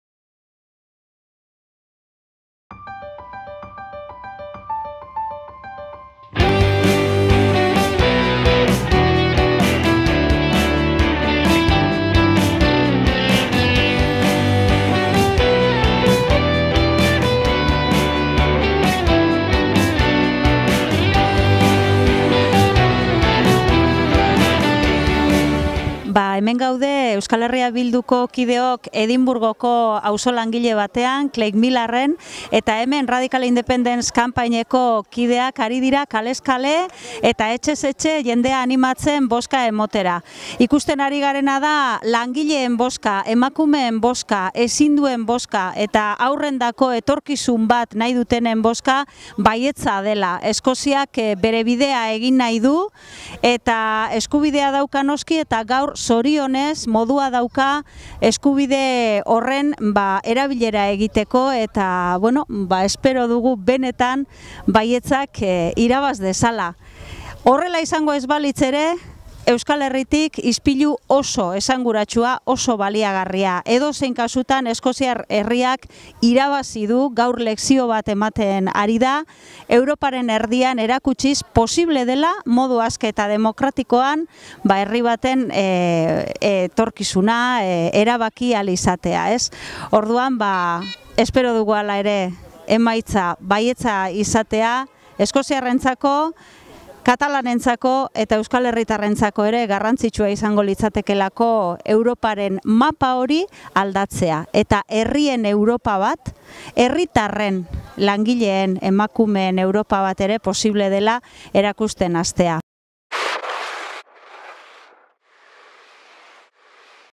Maribi Ugarteburu: "Langileen, emakumeen, ezinduen... bozka independentzia da"
Maribi Ugarteburuk oraindik bozketak egiten ari zirela egindako adierazpenak Edinburgotik.